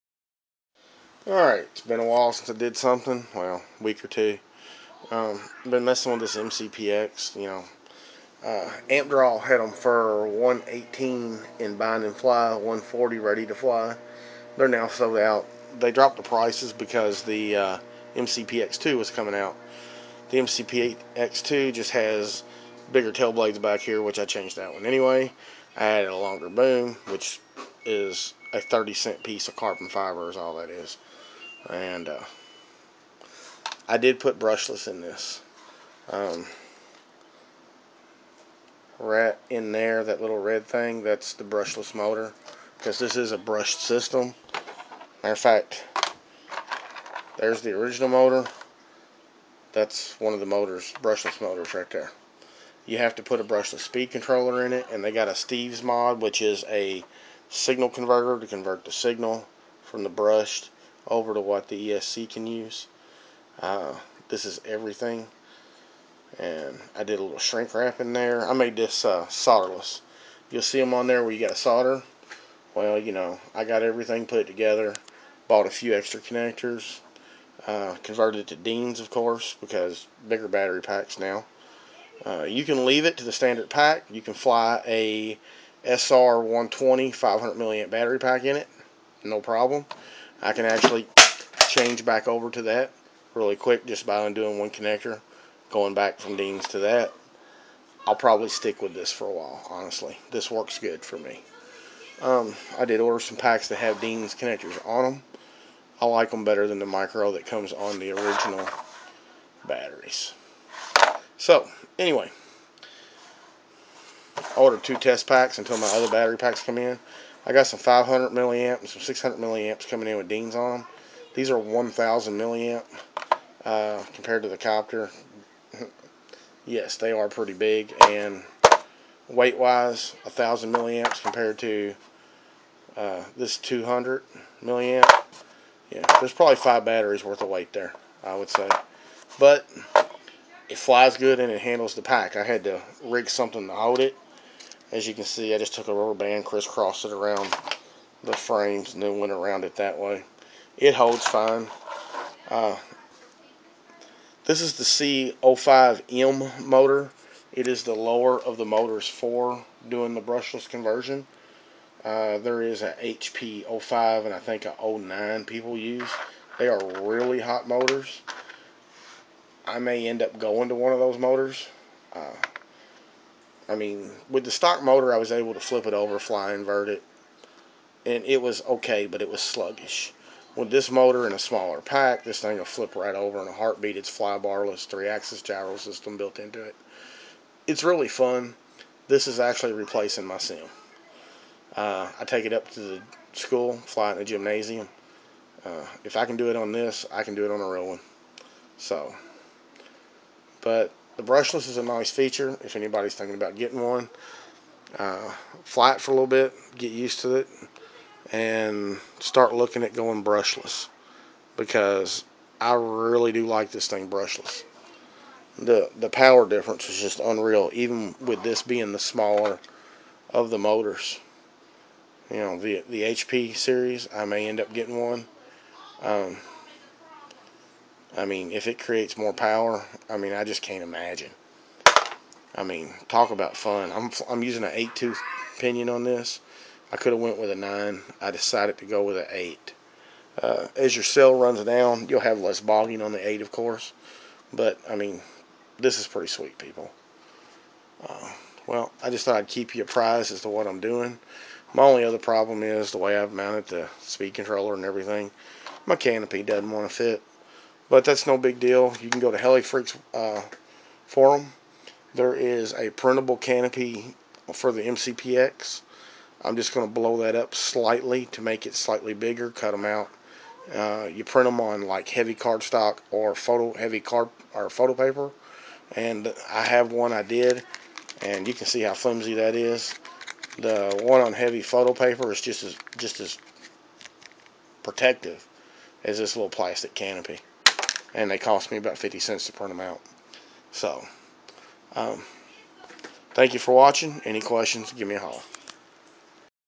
McpX brushless mod
McpX with brushless motor esc and steve converter installed
completely solderless to the 3 in 1 mcpx board